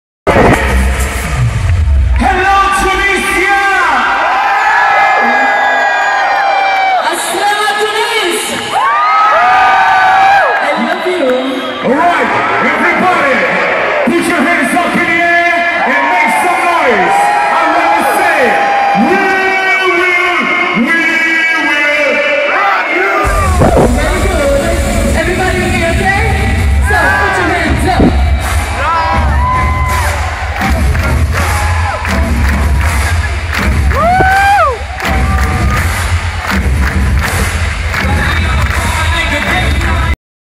White Sensation (Made In Tunisia) 17 By Aymen
White Sensation (Made in Tunisia) June, 21, 2012, With Tom Boxer & Morena, Akcent and Deepside Deejays. Amazing moments!